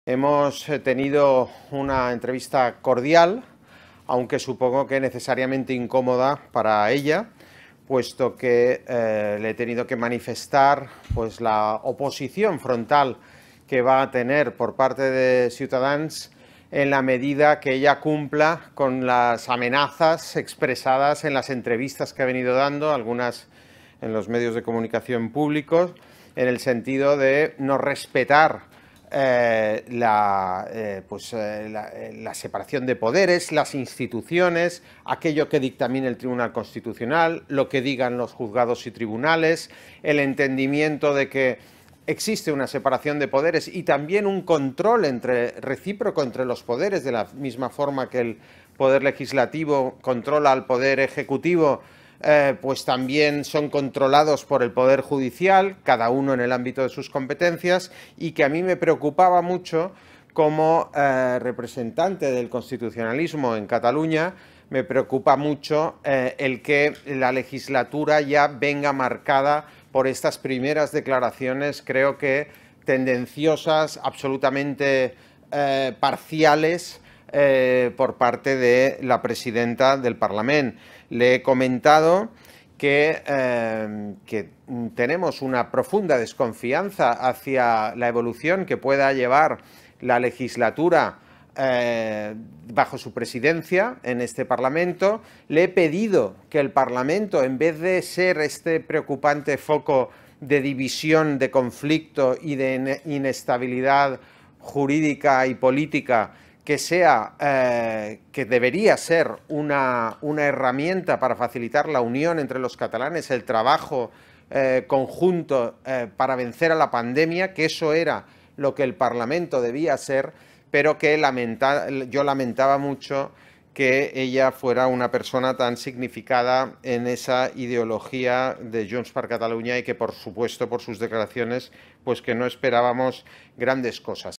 Carrizosa (Cs) declara su oposición a la investidura
El líder de Cs en Catalunya ha declarado después de la reunión con la presidenta del Parlament, Laura Borràs, que se va a oponer a la candidatura del presidente de la Generalitat. Hay dos posibles candidatos que se han postulado: Salvador Illa (PSC), que no tiene los apoyos suficientes para ser presidente, y Pere Aragonès (ERC), que podría ser investido con los votos de Junts y la CUP.
(Fuente: Parlament Catalunya)